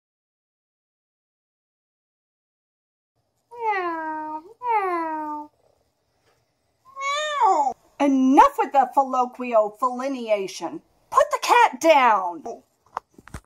Philloquiofeliniation: Cat Whispering (Gish 2018 Item 111)
SquidSquadLovesThanosSnap's entry for Gish 2018 Item 111 ("I’ve read the dictionary hundreds of times and frankly, it’s gotten predictable. It’s time for a sequel, and Gishers should write it! Coin a new word that is longer than 10 letters (the more the better!). The word must be lexically accurate - no-nonsense words or words that are really one long sentence all smashed together. You must be able to break your new word down into root words, and you must give the definition. Use it correctly in a sentence.")

philloquiofeliniation
\fi-lō-kwē-ō-fe-li-nē-āsh-ən\
noun
1. The action or process of speaking to cats
2. To be a cat whisperer
Origin: from Greek philos ‘loving.’ + Latin loqui ‘talk.’ + Latin feles ‘cat.’
Other forms: philloquiofeliniate (noun), philloquiofeliniating (verb)

Directed, filmed, and (crudely) edited by Jessa! Credit to Ayley (the cat whispererer), Anastasia (the anti-philloquiofeliniate), Oliver Cromwell (the innocent cat), and Maximilien Robespierre (the bystander cat)